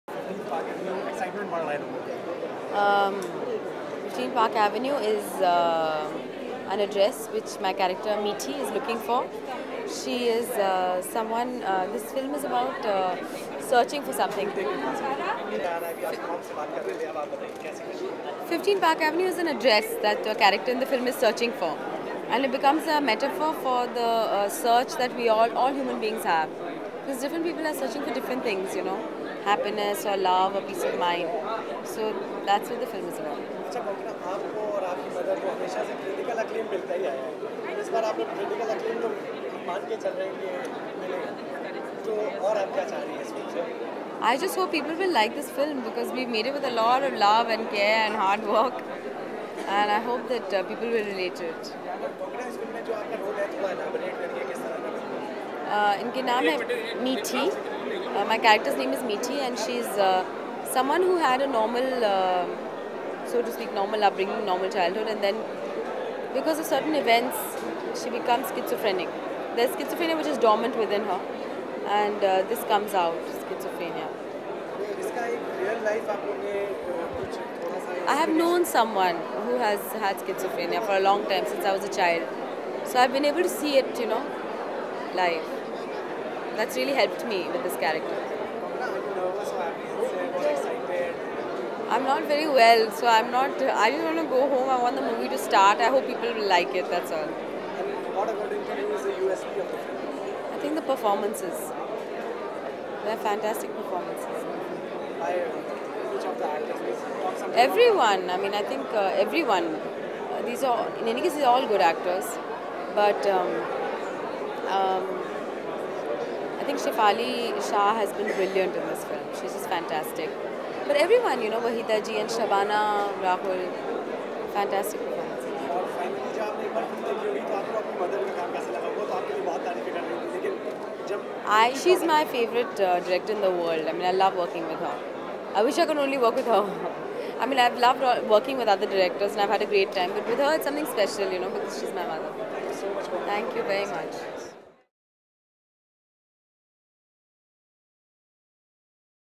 Konkona Sen: This film is about searching. Human beings search for happiness, love, peace of mind!
Konkona Sen Interview on her role in the film 15 Park Avenue, at the premiere: "My character Meethi is searching for this address. This film is about searching for something. It becomes a metaphor for the search we all human beings have. We human beings are searching for happiness, love, peace of mind. We have made it with a lot of love, care and hard work.m She becomes schizophrenic. It was dormant within her."

Konkona Sen Interview on her role in the film 15 Park Avenuem, at the premiere